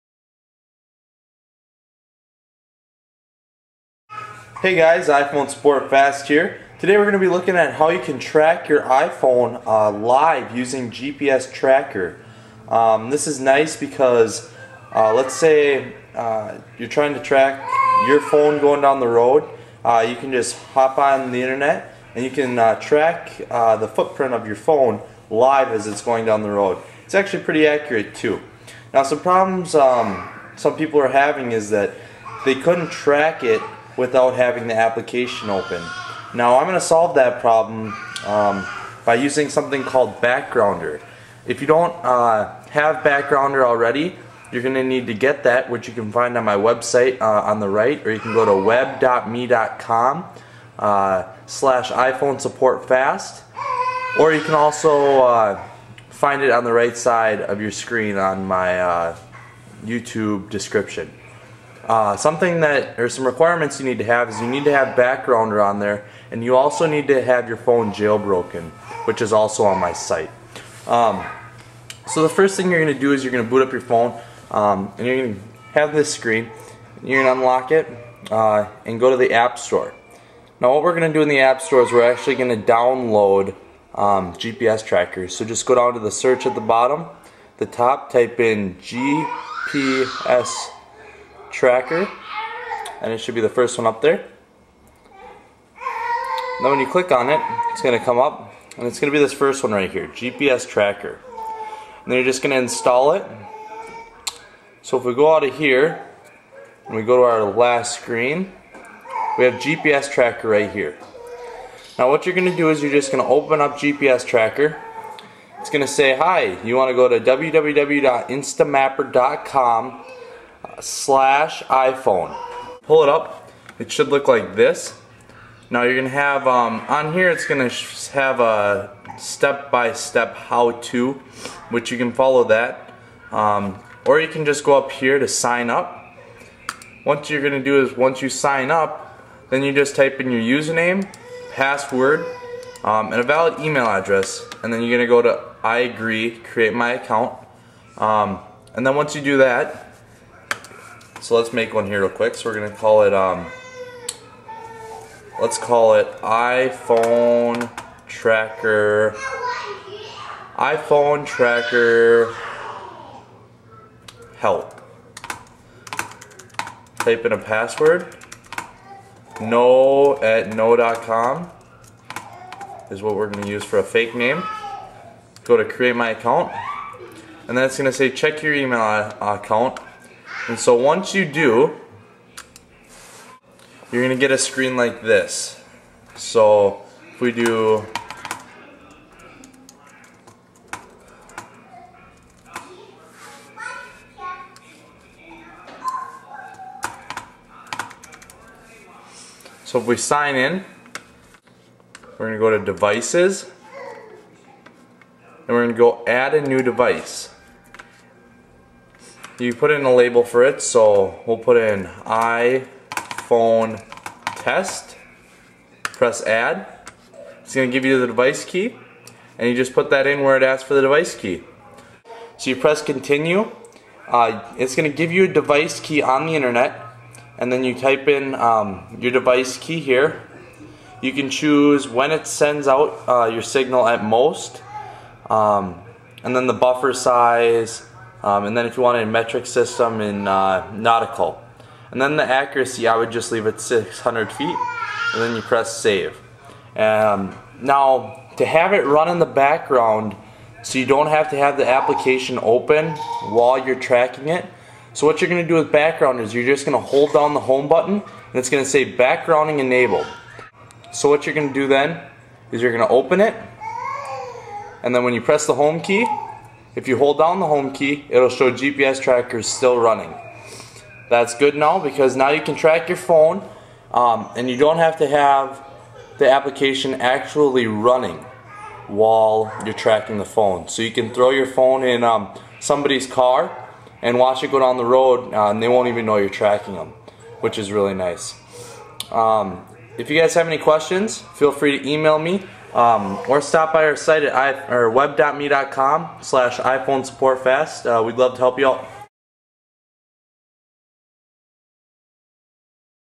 iPhone Tracking Live
How to track your iPhone live and keep record of it by iPhone Support Fast!